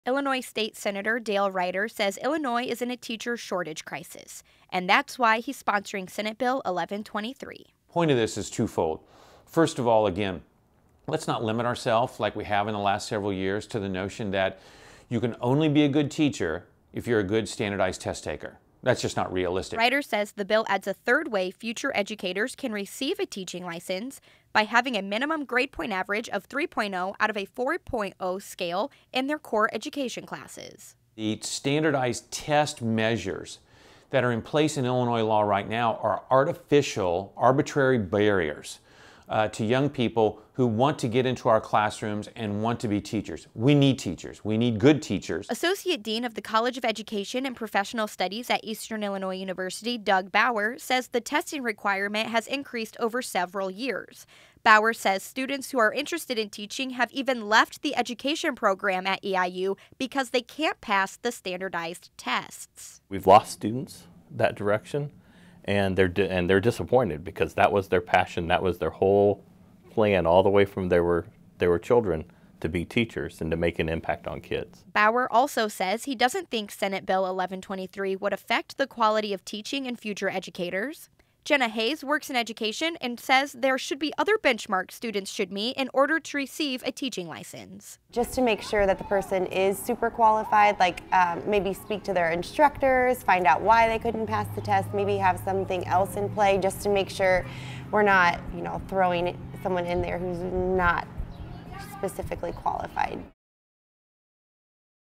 Illinois Teacher Shortage
If passed, Senate Bill 1123 would allow future teachers to receive a teaching license through a minimum grade point average of 3.0 out of a 4.0 scale in their core education classes.
Illinois State Senator Dale Righter said, Illinois is in a teacher shortage crisis, and that's why he's sponsoring Senate Bill 1123.
"First of all, let's not limit ourselves like we have in the last several years to the notion that you can only be a good teacher if you're a good standardized test taker. That's just not realistic."
Righter says the bill adds a third way future educators can receive a teaching license - by having a minimum grade-point-average of 3.0 out of a 4.0 scale in their core education classes.
"The standardized test measures that are in place in Illinois law right now are artificial, arbitrary barriers to young people who want to get into our classrooms and want to be teachers. We need teachers. We need good teachers."
Associate Dean of the College of Education and Professional Studies at Eastern Illinois University, Doug Bower said, the testing requirement has increased over several years.
Bower said students who are interested in teaching have even left the education program at EIU because they can't pass the standardized tests.
"We've lost students that direction, and they're disappointed because that was their passion. That was their whole plan all the way from when they were children, to be teachers, and to make an impact on kids."
Bower doesn't think Senate Bill 1123 would affect the quality of teaching in future educators.
Jenna Hays works in education and said there should be other benchmarks students should meet in order to receive a teaching license.
"Just to make sure the person is super qualified. Maybe speak to their instructors, find out why they couldn't pass the tests. Maybe have something else in play just to make sure we're not, you know, throwing someone in there who's not specifically qualified."
Righter saidthere are about 65-hundred un-filled teaching positions in Illinois for grades K through 12.
Righter hopes the bill will see action once lawmakers return from Spring Break April 25th.